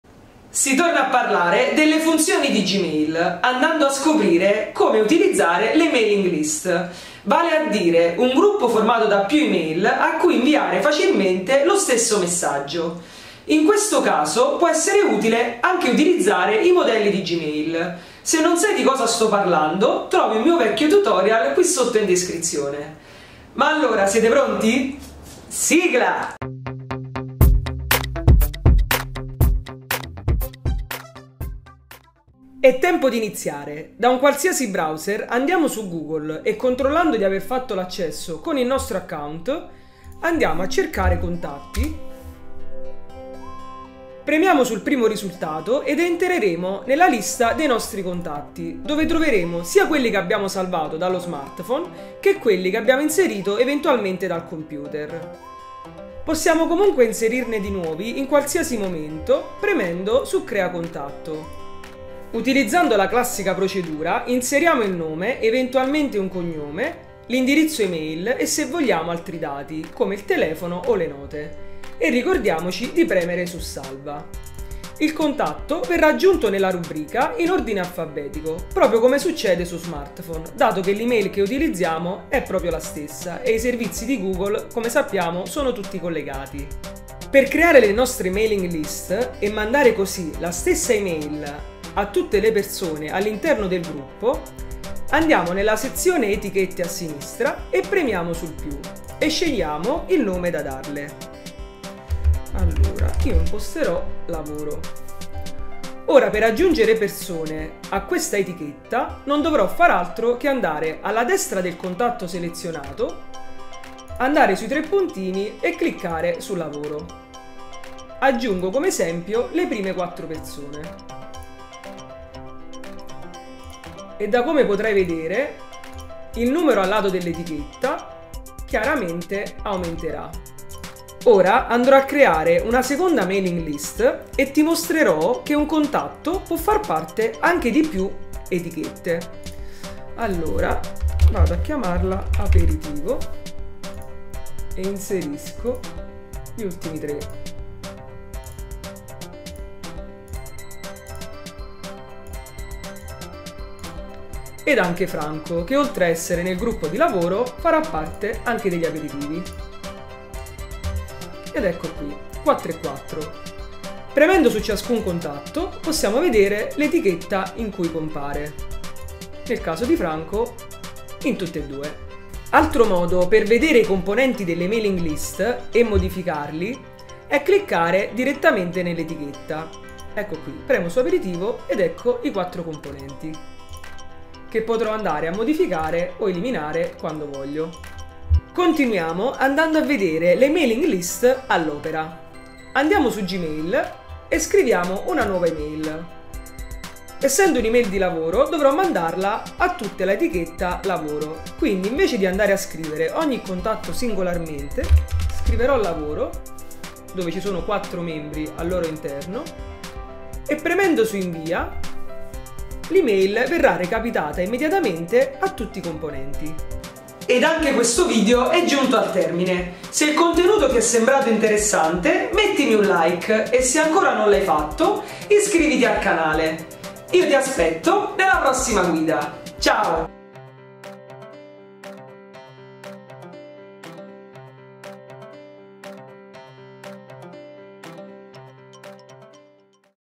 Come CREARE una MAILING LIST con GMAIL (gruppo di email) 👨‍👩‍👧‍👦
Il metodo per creare un'etichetta contenente più indirizzi email e mandare così lo stesso messaggio immediatamente a più persone 📮

❤️ Scrivi SIGLAAA nei commenti per dimostrarmi che hai visto il video e avrai un cuore!
☕️ Se vuoi supportarmi per il lavoro svolto, *offrimi un caffè* , te ne sarei davvero grato 😊